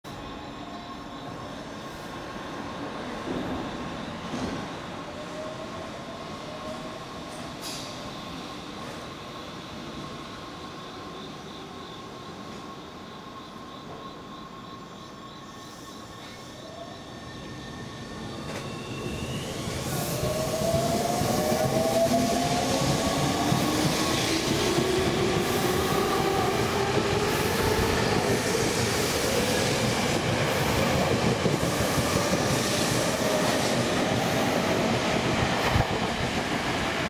３００００系東武東上線各停池袋行き志木駅発車その２
３１６０７Ｆ＋３１４０７Ｆ、２０１５．９．２７撮影です。